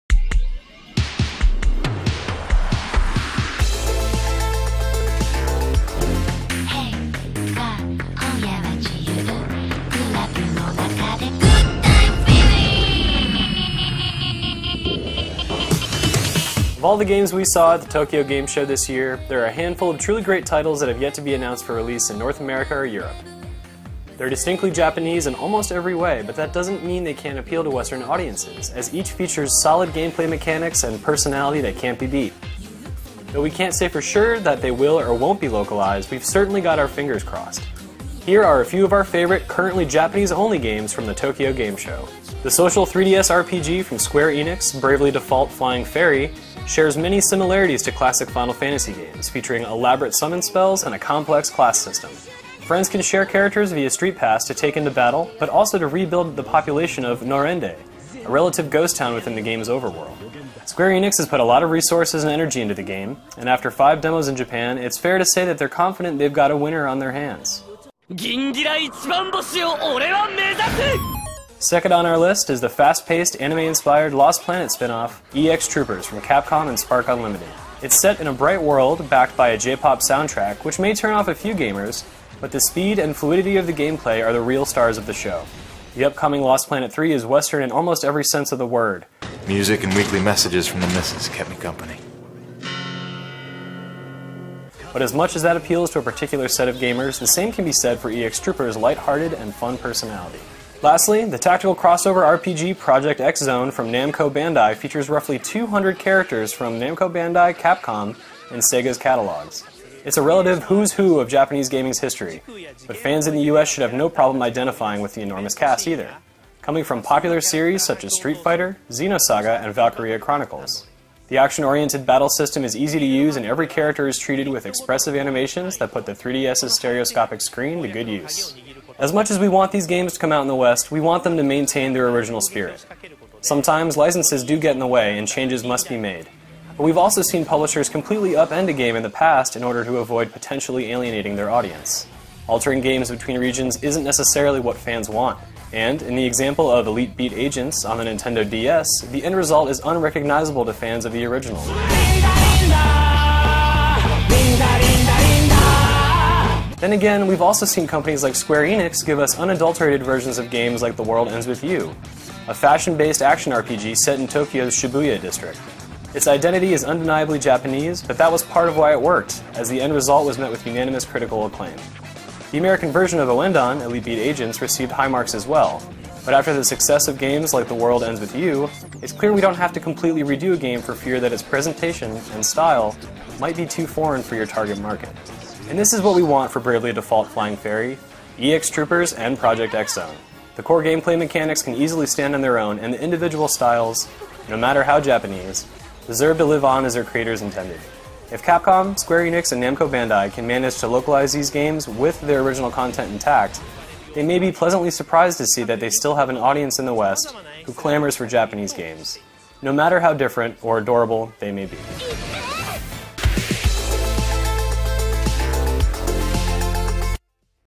Localize It - Japan, We Want These Ones!
Check out some of the most interesting games that are currently not slated for release outside of Japan.